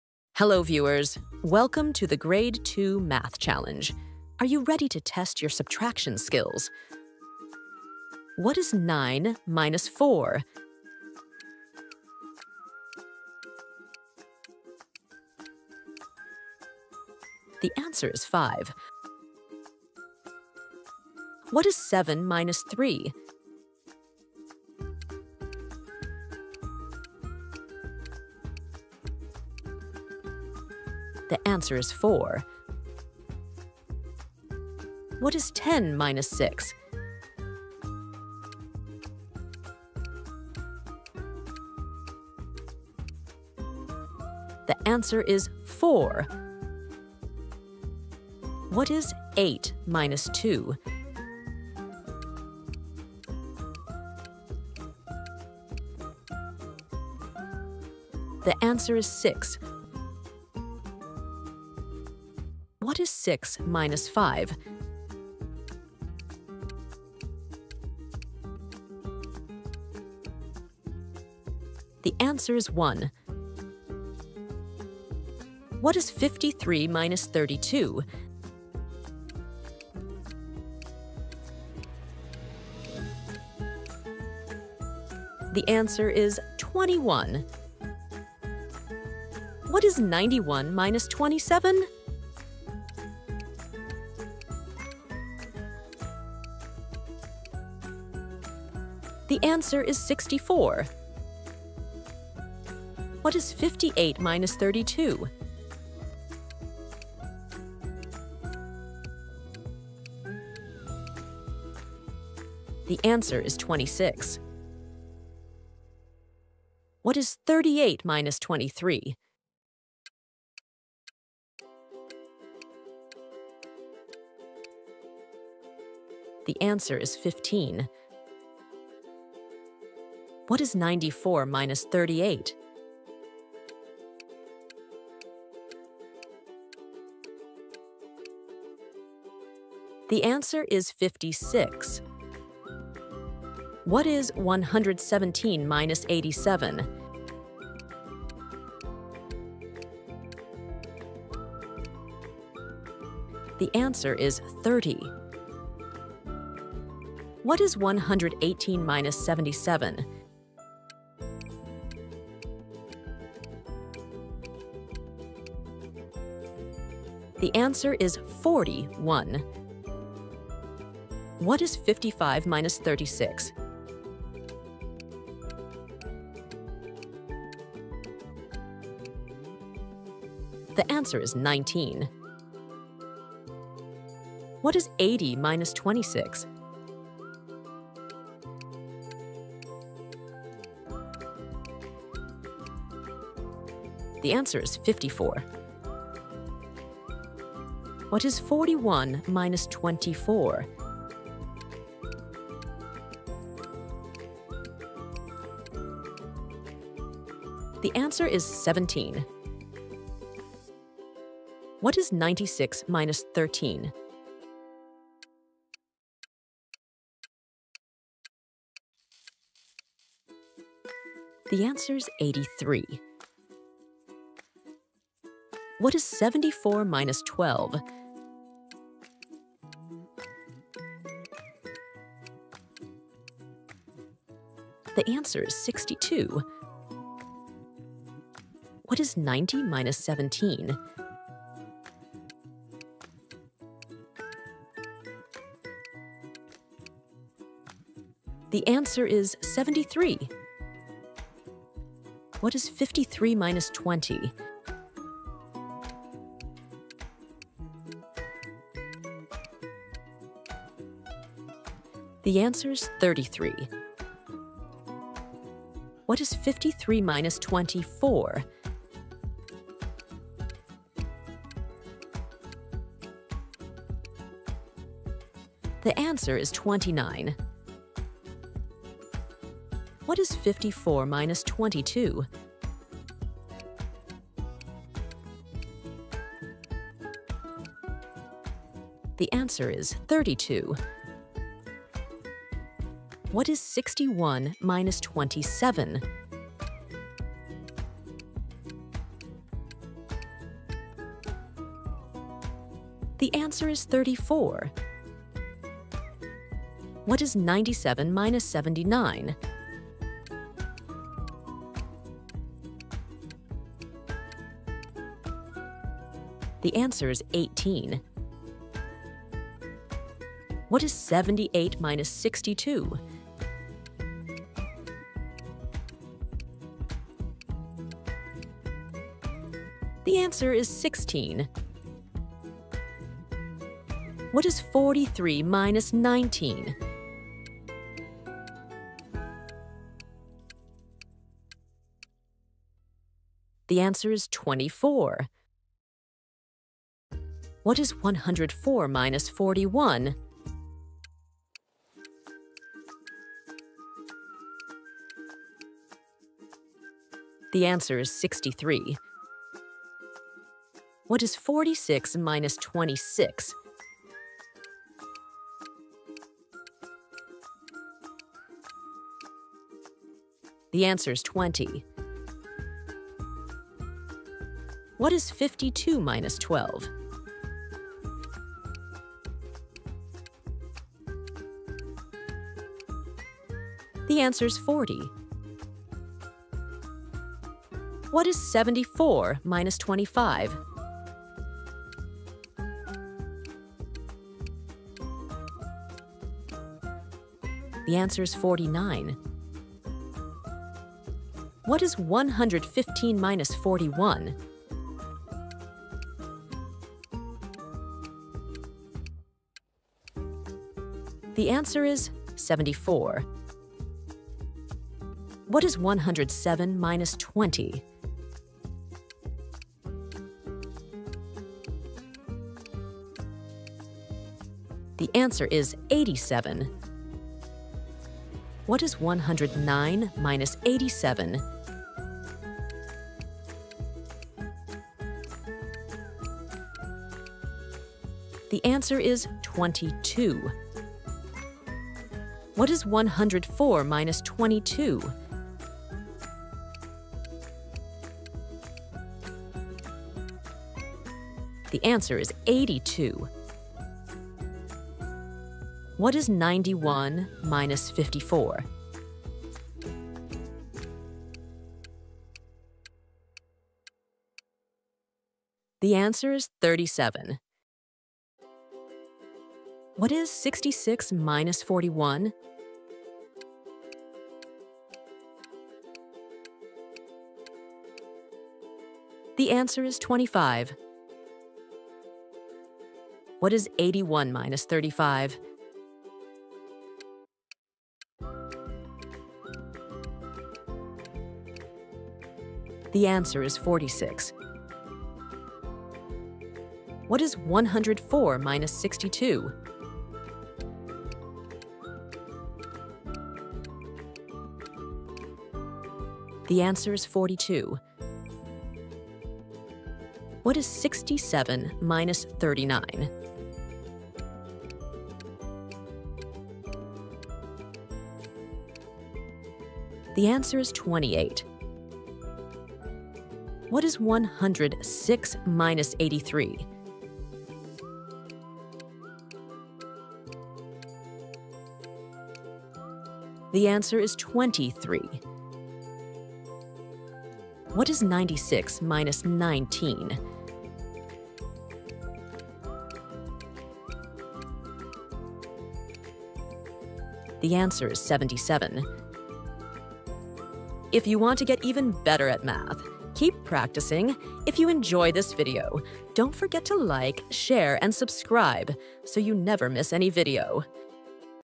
Grade 2 Math's Subtraction for Kids | Easy 2-Digit Math Practice
Let’s practice Grade 2 subtraction in a simple and easy way! 🧮 This math video is perfect for helping kids learn how to subtract 2-digit numbers, with and without borrowing (regrouping).

Clear instructions, step-by-step examples, and practice problems make this video a great tool for students learning at home or in school.

✨ What’s Included:
✔️ 2-digit subtraction
✔️ Borrowing explained simply
✔️ Practice problems with answers
✔️ Clear visuals and voice guidance

🎓 Best For:

Grade 2 students (Ages 7–8)

Math homework help

Classroom & homeschool learning

Daily subtraction practice